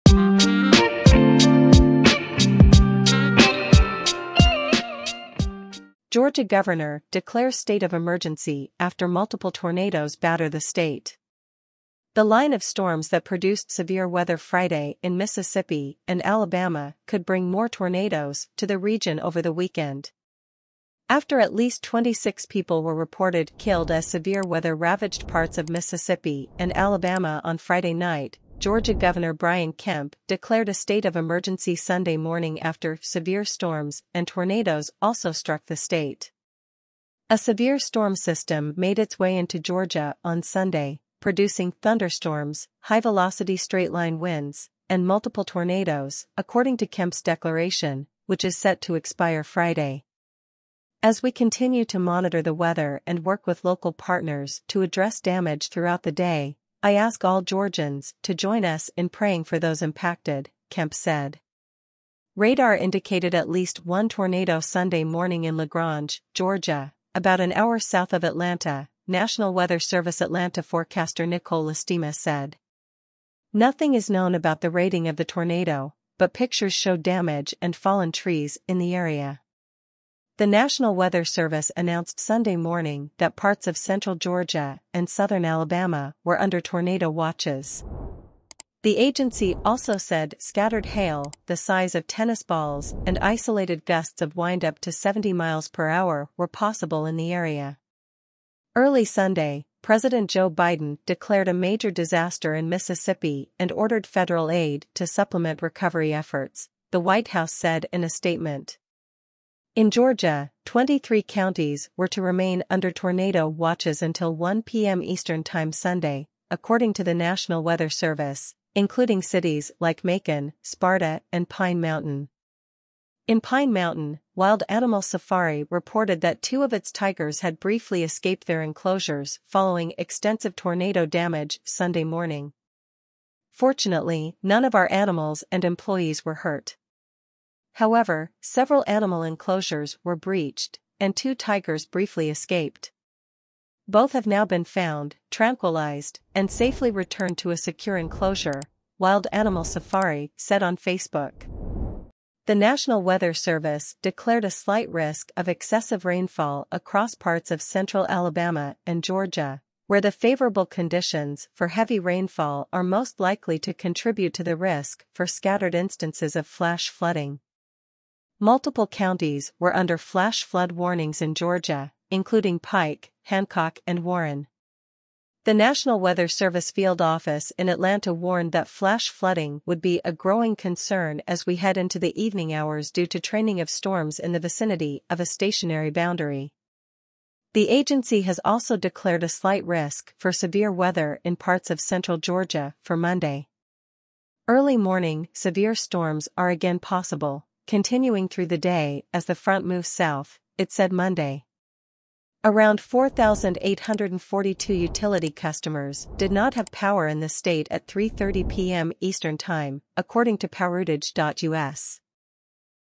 Georgia governor declares state of emergency after multiple tornadoes batter the state#news
Georgia governor declares state of emergency after multiple tornadoes batter the state

The line of storms that produced severe weather Friday in Mississippi and Alabama could bring more tornadoes to the region over the weekend.
After at least 26 people were reported killed as severe weather ravaged parts of Mississippi and Alabama on Friday night, Georgia Gov. Brian Kemp declared a state of emergency Sunday morning after "severe storms and tornadoes" also struck the state.

A severe storm system made its way into Georgia on Sunday, producing "thunderstorms, high-velocity straight-line winds, and multiple tornadoes," according to Kemp's declaration, which is set to expire Friday.

"As we continue to monitor the weather and work with local partners to address damage throughout the day, I ask all Georgians to join us in praying for those impacted," Kemp said.

Radar indicated at least one tornado Sunday morning in LaGrange, Georgia, about an hour south of Atlanta, National Weather Service Atlanta forecaster Nikole Listemaa said.
Nothing is known about the rating of the tornado, but pictures show damage and fallen trees in the area.

The National Weather Service announced Sunday morning that parts of central Georgia and southern Alabama were under tornado watches. The agency also said scattered hail the size of tennis balls and isolated gusts of wind up to 70 mph were possible in the area.
Early Sunday, President Joe Biden declared a major disaster in Mississippi and ordered federal aid to supplement recovery efforts, the White House said in a statement.

In Georgia, 23 counties were to remain under tornado watches until 1 p.m. ET Sunday, according to the National Weather Service, including cities like Macon, Sparta and Pine Mountain.

In Pine Mountain, Wild Animal Safari reported that two of its tigers had briefly escaped their enclosures following "extensive tornado damage" Sunday morning.
"Fortunately, none of our animals and employees were hurt. However, several animal enclosures were breeched, and two tigers briefly escaped. Both have now been found, tranquilized, and safely returned to a secure enclosure," Wild Animal Safari said on Facebook.
The National Weather Service declared a slight risk of excessive rainfall across parts of central Alabama and Georgia, "where the favorable conditions for heavy rainfall are most likely to contribute to the risk for scattered instances of flash flooding."

Multiple counties were under flash flood warnings in Georgia, including Pike, Hancock and Warren.

The National Weather Service field office in Atlanta warned that flash flooding would be "a growing concern as we head into the evening hours due to training of storms in the vicinity of a stationary boundary."

The agency has also declared a slight risk for severe weather in parts of central Georgia for Monday.
Early morning severe storms are again possible, continuing through the day as the front moves south," it said Monday.

Around 4,842 utility customers did not have power in the state at 3:30 p.m. ET, according to poweroutage.us.